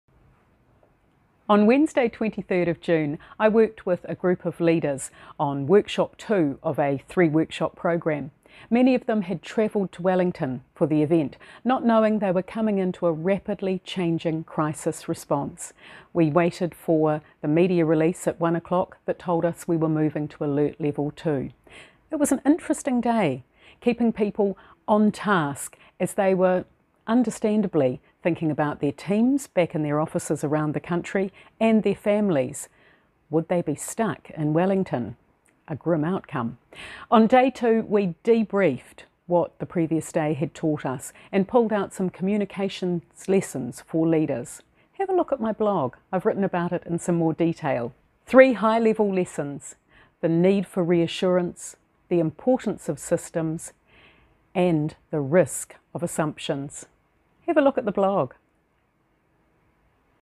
Real-time learning about change communication
A change in pandemic response alert levels In Wellington gave Dinah the chance to deliver some real-time learning about change communication in June 2021. She previews her blog on the lessons from the day